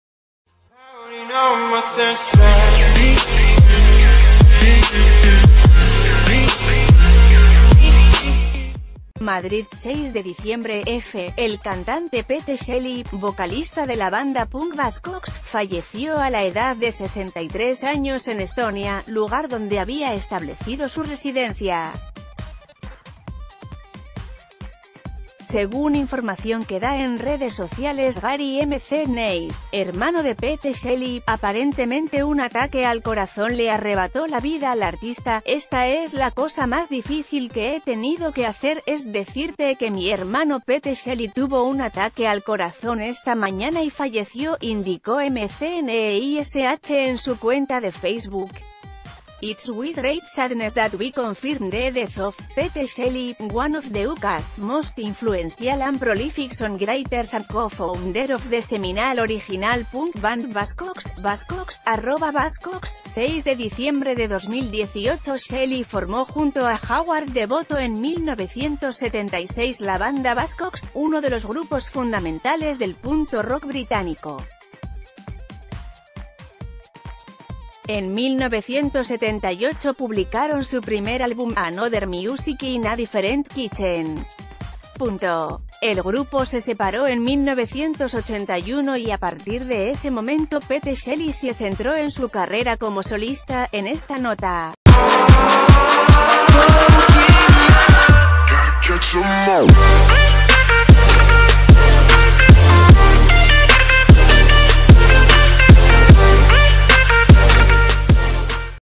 Fallece Pete Shelley, fundador del grupo Buzzcocks
Madrid, 6 dic (EFE).- El cantante Pete Shelley, vocalista de la banda punk Buzzcocks, falleció a la edad de 63 años en Estonia, lugar donde había establecido su residencia.